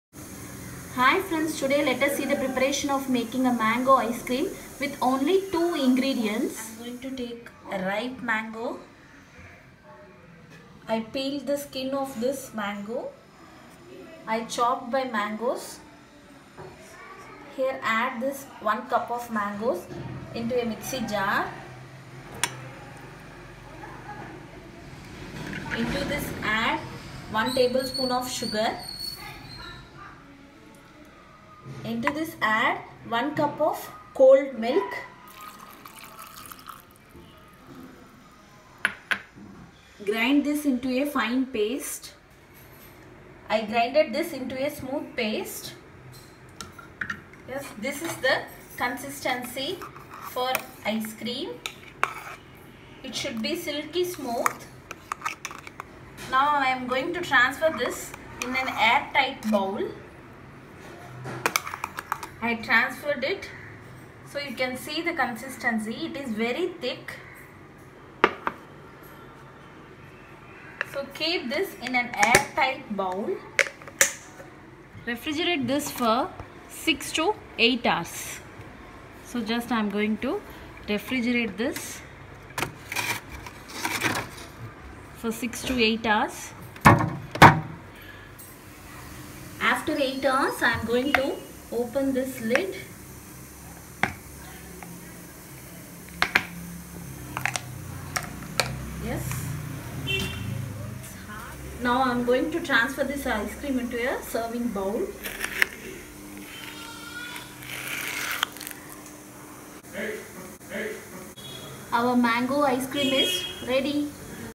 Mango Ice Cream
An ice cream with only milk , sugar and mango .A perfect treat for summertime